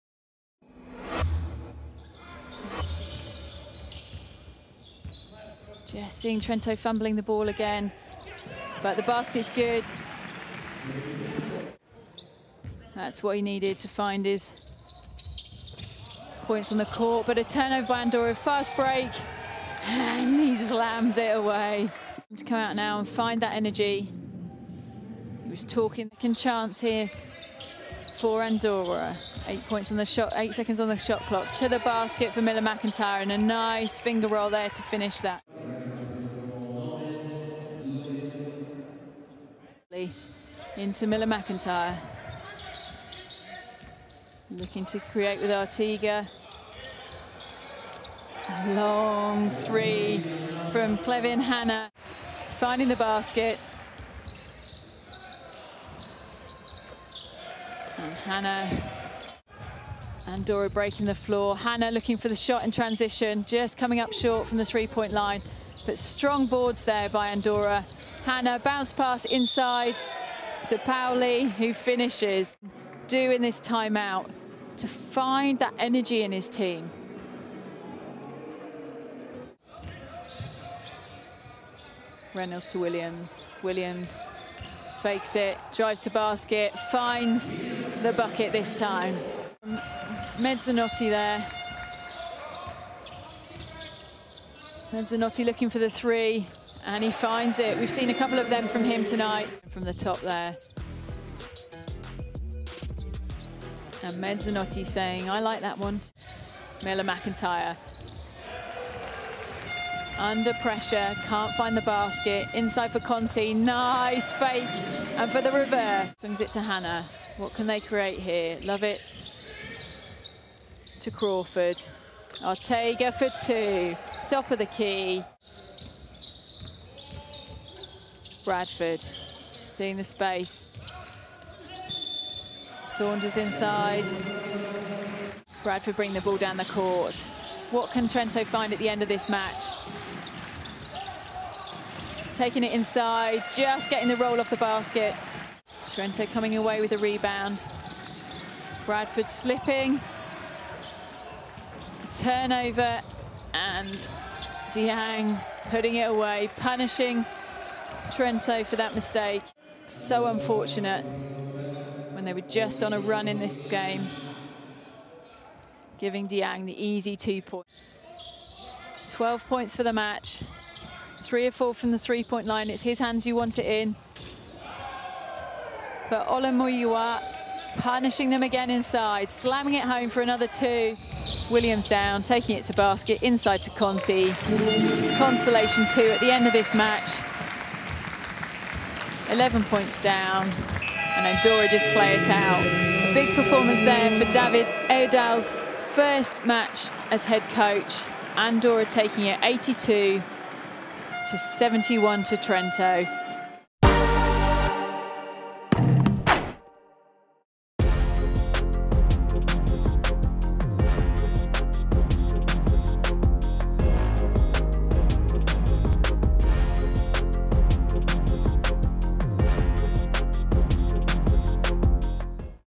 Andorra snaps losing streak in Trento! | Round 11, Highlights | 7DAYS EuroCup
Morabanc Andorra returned to winning ways in the 7Days Eurocup by beating Dolomiti Energia Trento 71-82 at BLM Group Arena on Wednesday night. Drew Crawford set the tone for the winners with 15 points, Clevin Hannah added 12 and Nacho Llovet nailed 10.  Wesley Saunders led Trento posting 13 points, while Andrea Mezzanotte has 12 apiece.